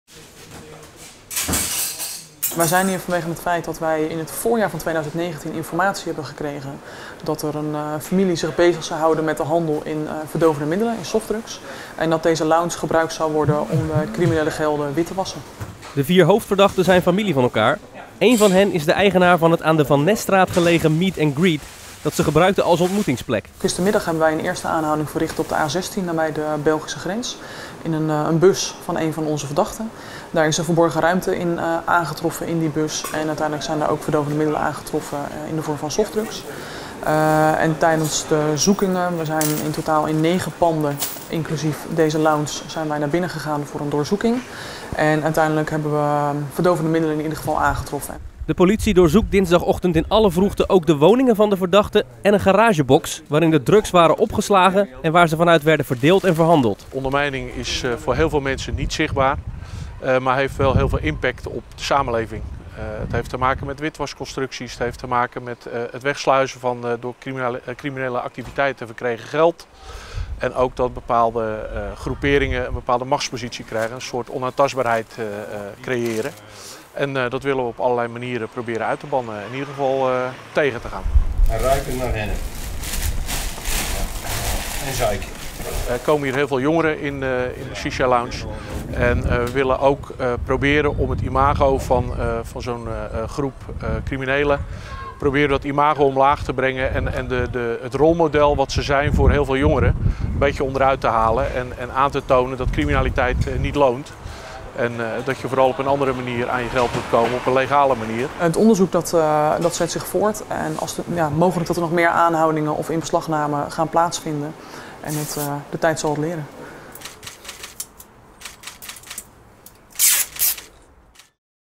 Zeven verdachten van drugshandel in Alphen aan den Rijn opgepakt
Zeven verdachten uit Alphen aan den Rijn zijn opgepakt in een onderzoek naar drugshandel, witwassen en deelname aan een criminele organisatie. Daarnaast is op last van de gemeente Alphen shishalounge Meet & Greet aan de Van Nesstraat voor onbepaalde tijd gesloten. De politie vermoedt dat via het café met drugshandel verdiend geld werd witgewassen. Het onderzoek heeft maanden geduurd.